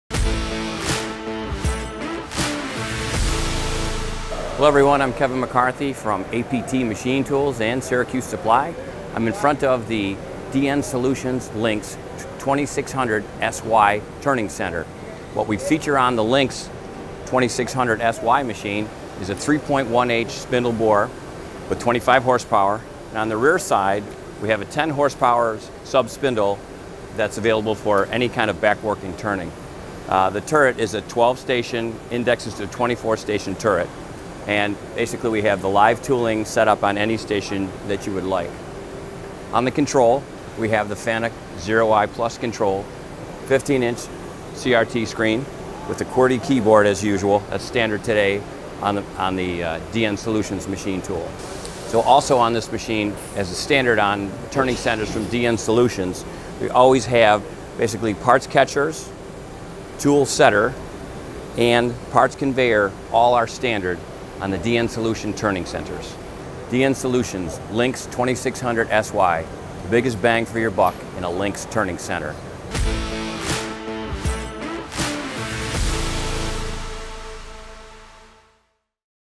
DN Solutions Lynx 2600SY (Formerly Doosan)
Syracuse Supply is your premiere source for CNC machinery, machine tool accessories and supplies.

Introducing the Lynx 2600SY, the largest model in DN Solutions' bestselling turning center series. If you need more size from your turning center but don’t need to jump up to the PUMA series, the 2600SY has your name on it.

CAPACITY
Main Spindle Std Chuck 255 mm (10")
Main Spindle Max Chuck 305 mm (12")
Max Turn Length 610 mm
Max Turn Diameter 380 mm
Bar Capacity 81 mm

TRAVELS
Guideway Type Linear
X Axis Rapid Traverse 30 m/min
Z Axis Rapid Traverse 30 m/min
Y Axis Rapid Traverse 10 m/min
Swing Over Bed 630 mm

MAIN SPINDLE
Main Spindle Power 18.5 kW
Spindle RPM 3,500 RPM
Spindle Torque 403 Nm
Spindle Bore 91 mm
Draw Tube ID 82 mm

SUB SPINDLE
Sub Spindle Power 7.5 kW
Sub Spindle RPM 4,500 RPM
Sub Spindle Drive Type Belt Drive

TURRET
Tool Mount Type BMT-55P
Turret Stations 12 (24 positions)
Rotary Tool RPM 5,000 RPM
Rotary Tool Motor 5.5 kW

DIMENSIONS
Machine Height 2,095 mm
Machine Length 3,425 mm
Machine Width 1,920 mm
Machine Weight 6,000 kg